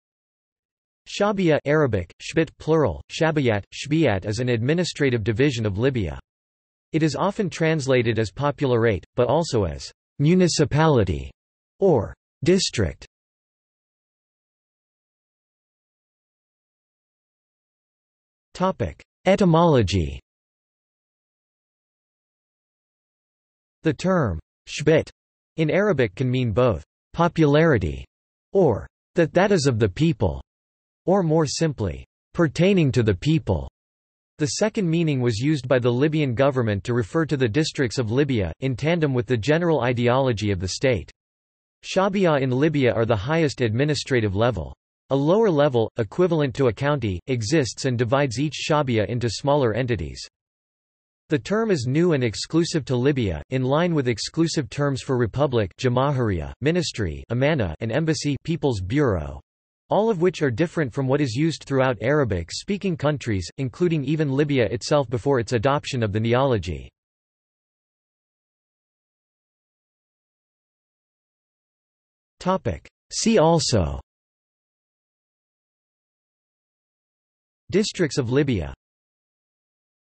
Shabiyah | Wikipedia audio article
This is an audio version of the Wikipedia Article:
Shabiyah

00:00:14 undefined
00:01:08 undefined

- Socrates

SUMMARY
Shabiyah (Arabic: شعبية‎) (plural: Shabiyat, شعبيات) is an administrative division of Libya. It is often translated as popularate, but also as "municipality" or "district".